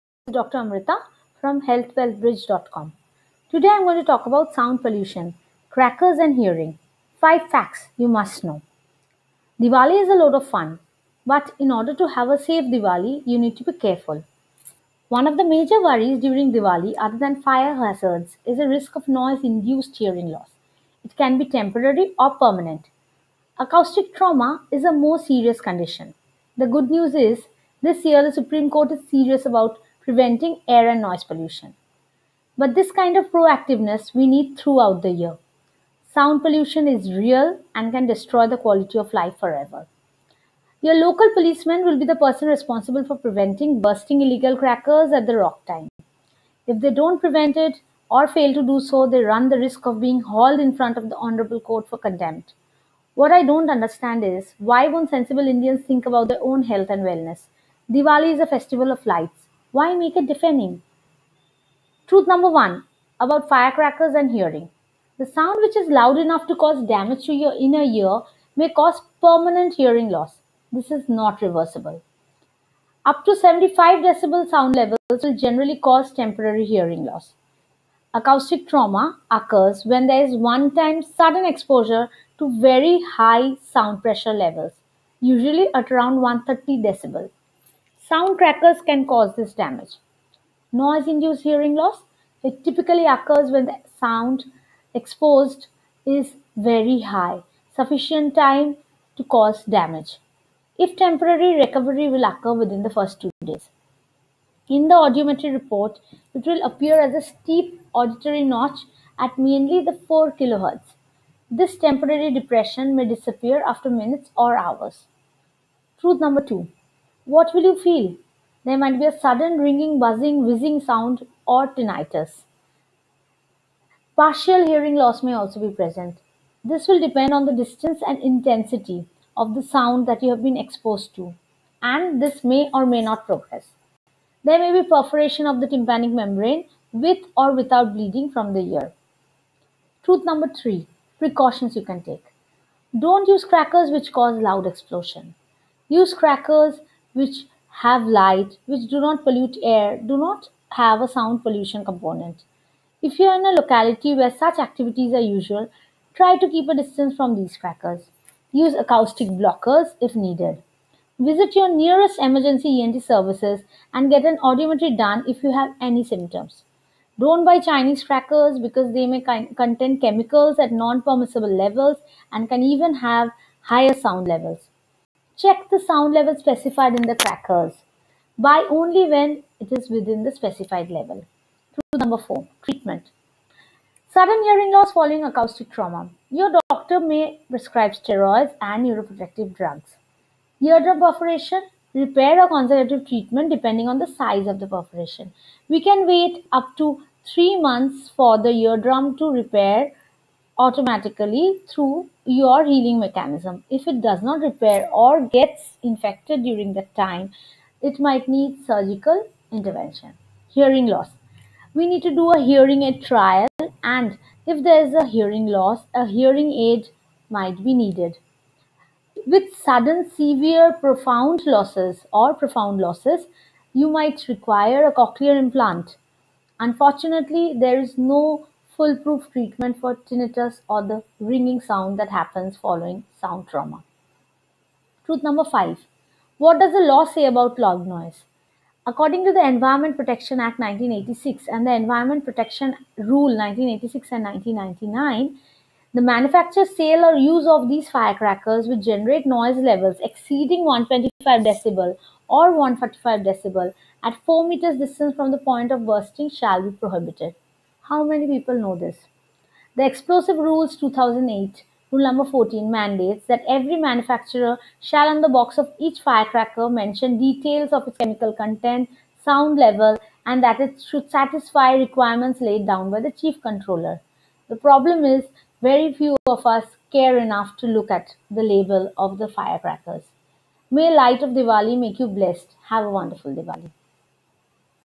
Noise Pollution Crackers & Hearing Loss :Effect|Symptoms|Prevention|Environment
Sound pollution happens throughout the year and it's real. Do you know why you are at a real risk of deafness and tinnitus with noise pollution?
What happens with a sudden severe acoustic injury? What are the symptoms?
With sudden noise trauma, you may have sudden deafness, ringing sound in the ear, dizziness and even perforation of the eardrum.
The good news is many recovers after this.
The bad news is with severe enough trauma, many suffer permanent damage.

Read this blog post for more info because you really don't want this to happen to anyone!